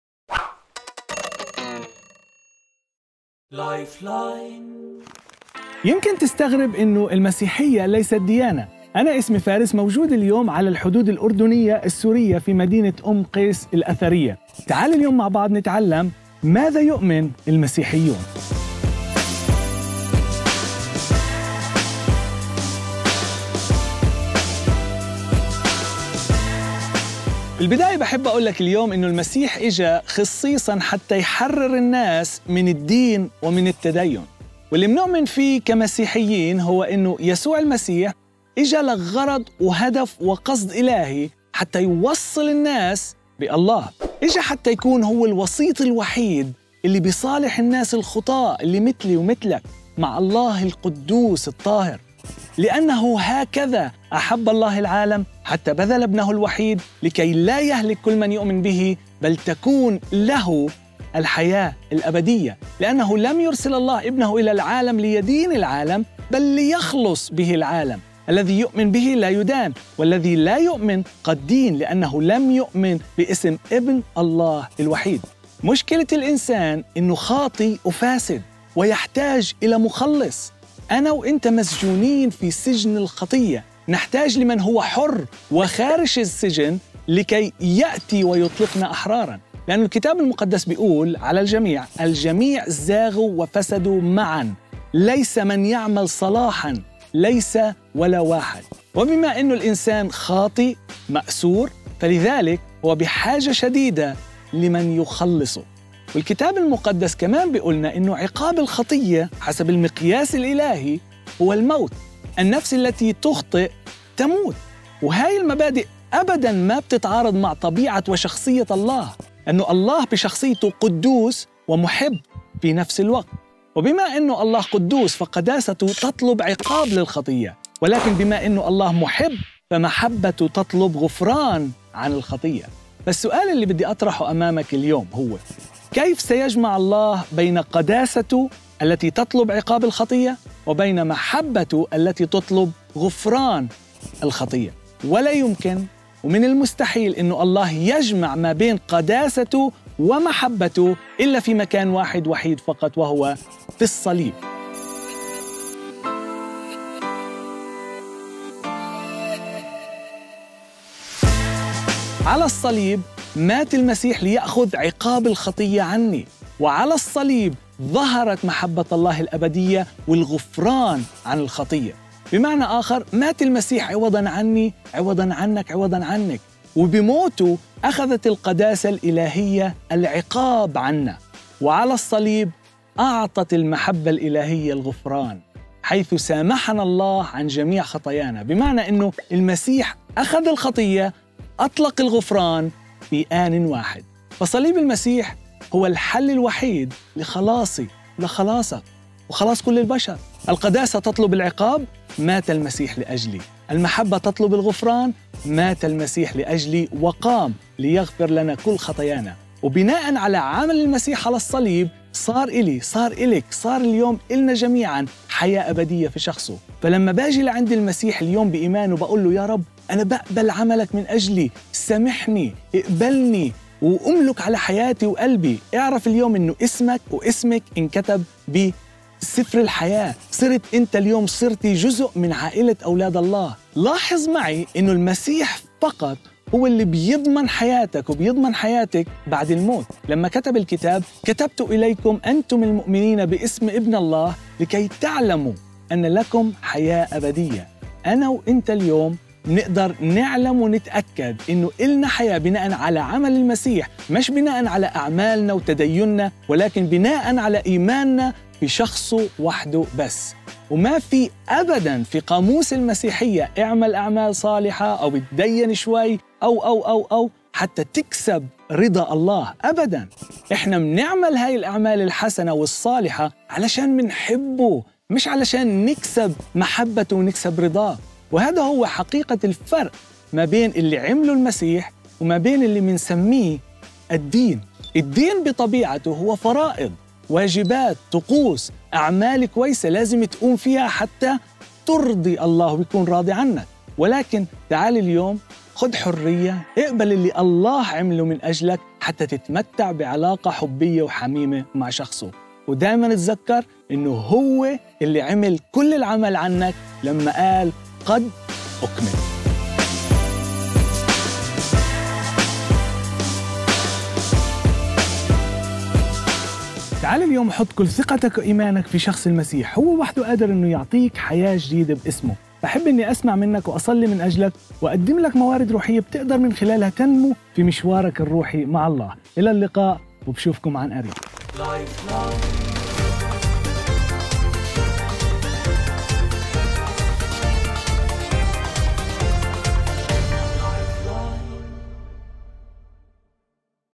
lifeline: المسيحية ليست ديانة
"أنا و أنتَ مسجونين في سجن الخطيّة، نحتاج لشخص حرّ لكي يطلقنا أحرار ..." شاهد هذا الوصف الجميل لما فعلّهُ الله من أجلك.

Produced by NEXTGEN Media. Visit us at nextgenarabic.com